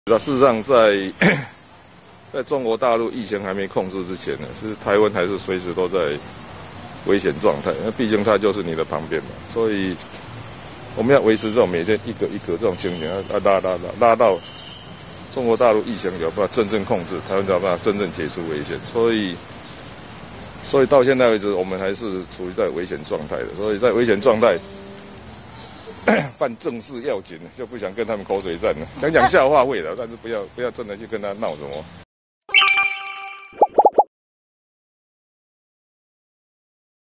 柯文哲：大陸疫情沒控制前 台灣隨時都在危險狀態
記者 張榮恩 拍攝